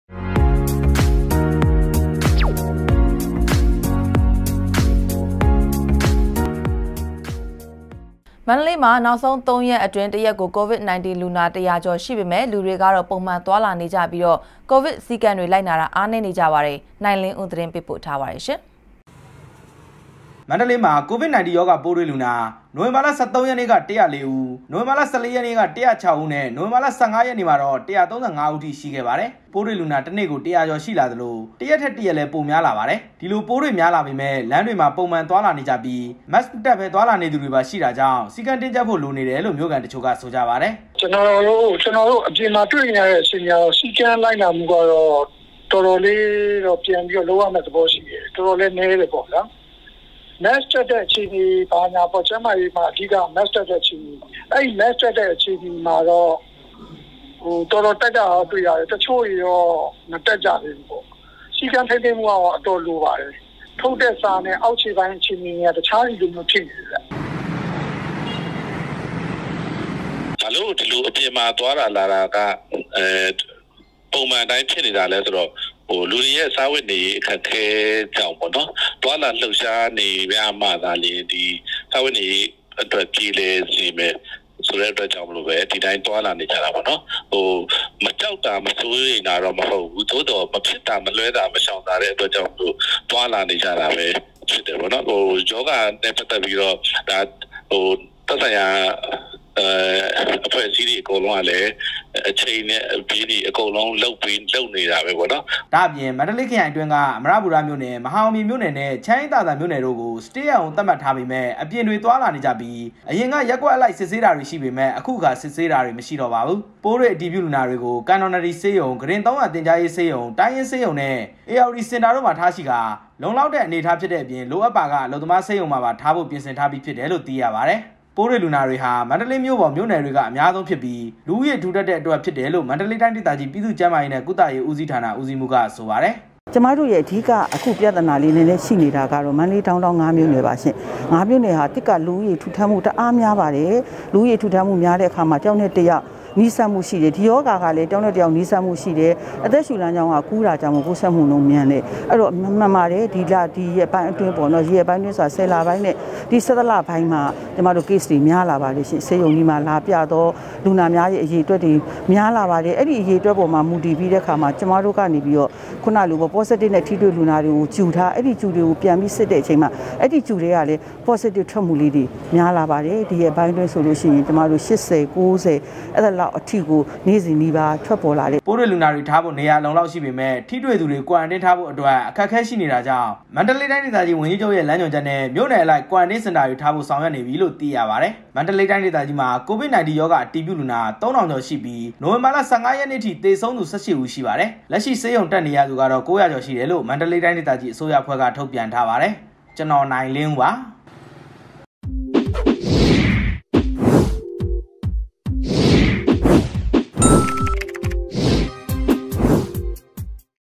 DVB - မန္တလေးမှာ ကိုဗစ်စည်းကမ်းတွေ လိုက်နာတာ အားနည်း
မန္တလေးမှာ နောက်ဆုံး (၃) ရက်အတွင်း တစ်ရက်ကို COVID - 19 လူနာ (၁၀၀) ကျော်ရှိပေမယ့် လူတွေကတော့ ပုံမှန်သွားလာနေကြပြီး ကိုဗစ်စည်းကမ်းတွေ လိုက်နာတာ အားနည်းကြပါတယ် ။ နိုင်လင်းဦး သတင်းပေးပို့ထားပါတယ်